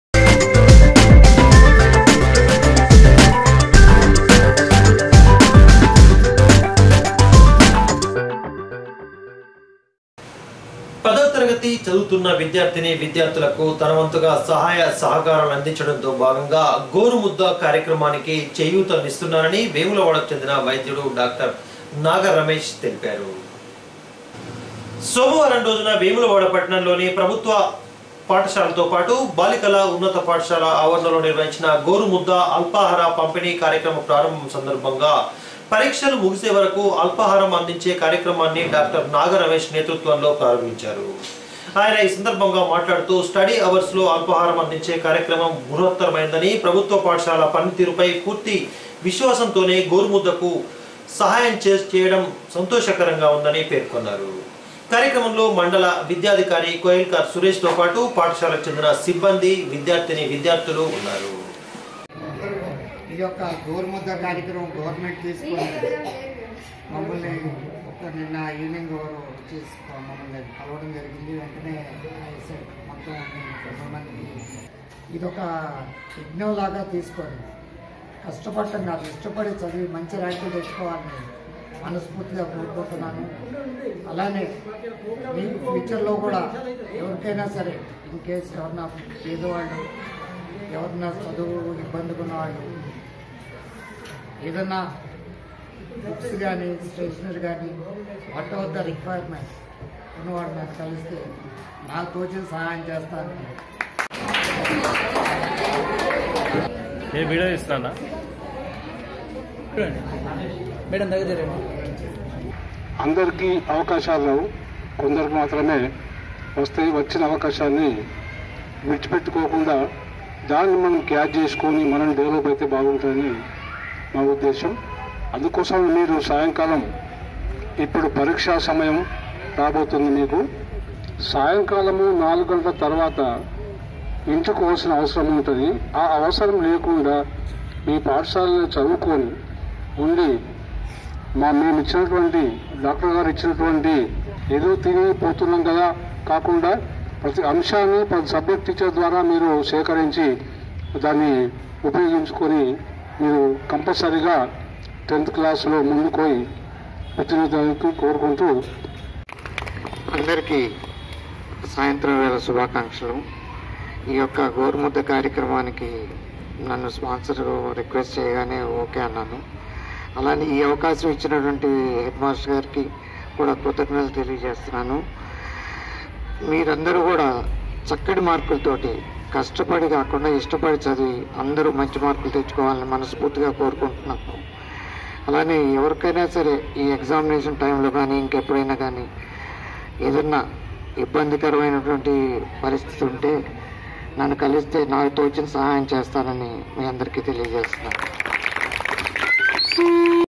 వేములవాడ ప్రభుత్వ పాఠశాలలో డా|| నాగ రమేష్ సహకారముతో గోరు ముద్ద కార్యక్రమం ప్రారంభం | సిటీ న్యూస్
సిటీ న్యూస్ | వేములవాడ